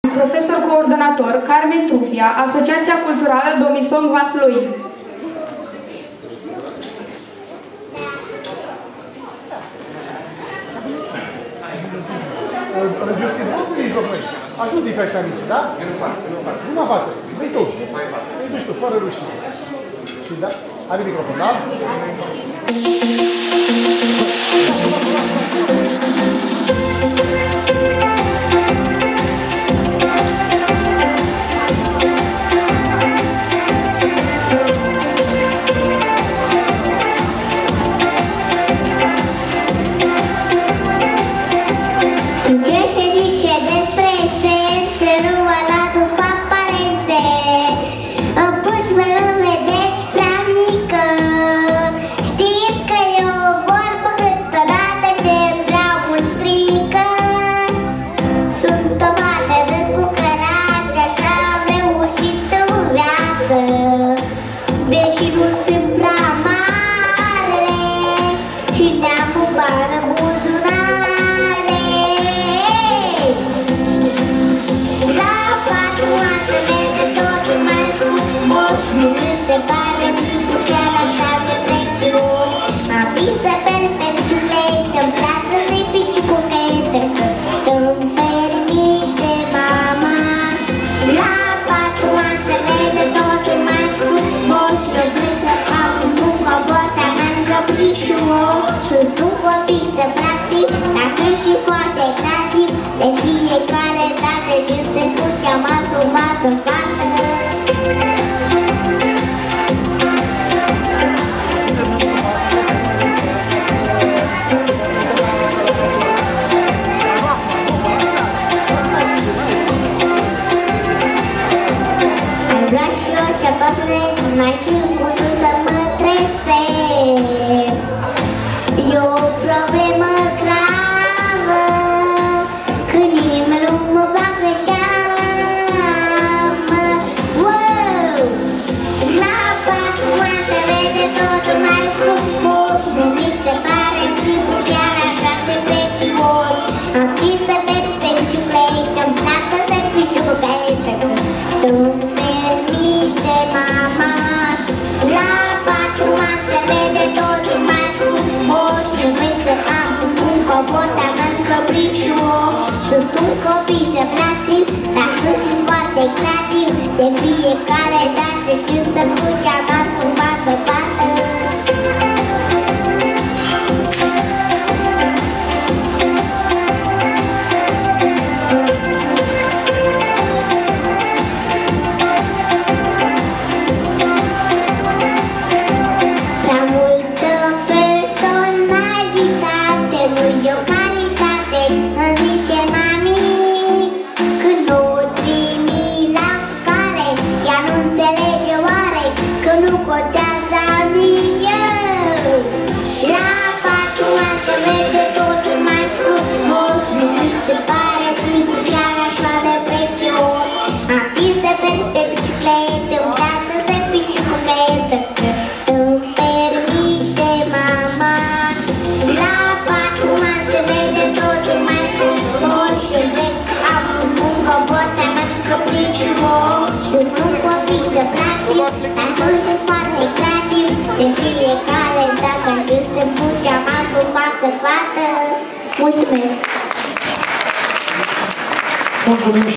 ILINCA CHIRATCU
PREMIUL II
FESTIVALUL NATIONAL DE MUZICA
"SUPERVEDETE DE DRAGOBETE", VASLUI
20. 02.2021